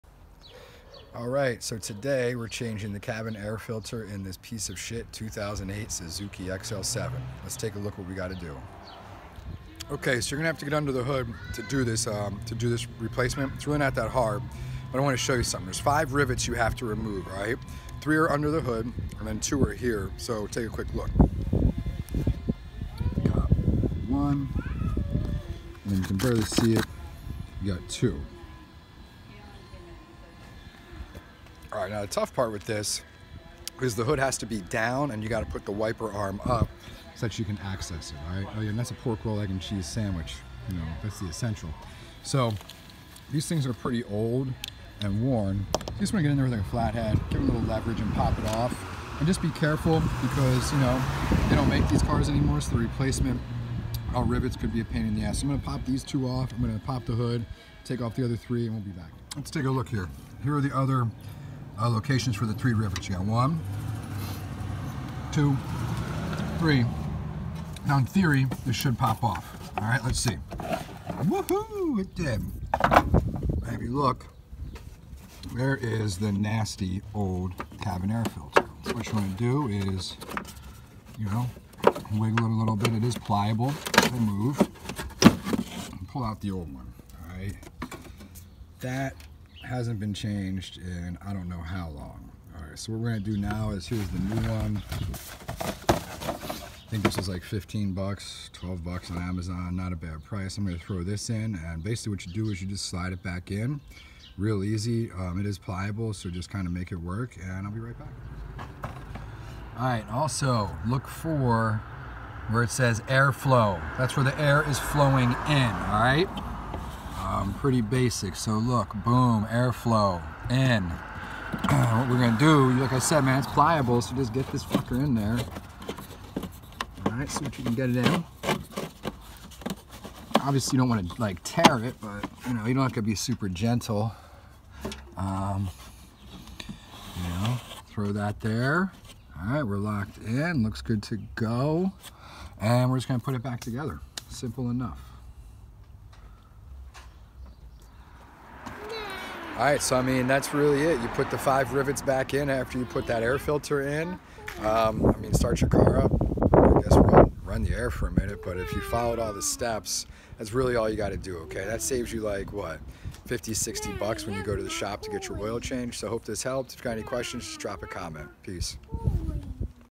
‘08 Suzuki XL-7 Cabin Air Filter Replacement
Allright, back with another installment of how to do something on the lovely 2008 XL-7!
Today I'm swapping out the cabin air filter after about 2 years of not doing so.
Not a good idea. Make sure you swap it out 1x a year to ensure clean air entering your vehicle.
Also, the filter is NOT behind the glove box.
It is under the hood.
So follow these steps and hopefully save yourself some $$!!

Link to purchase cabin air filter